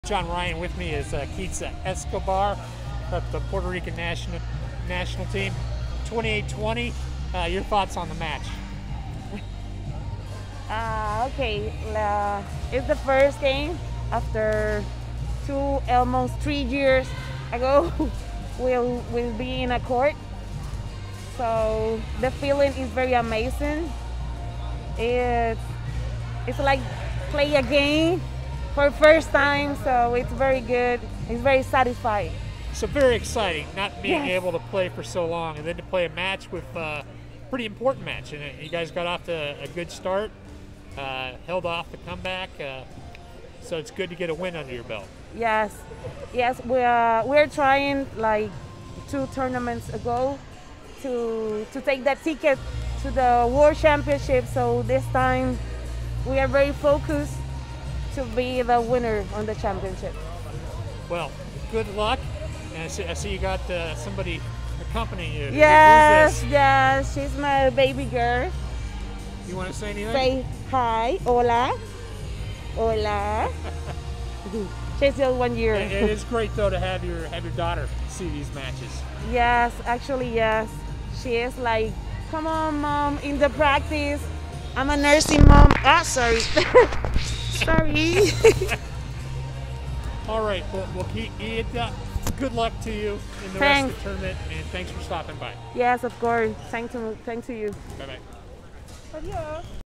Postgame Interview with Puerto Rico GK, Kitsa Escobar.
Interview after the USA-PUR match on 22 August, 2021 at the NACHC Women's Handball Match.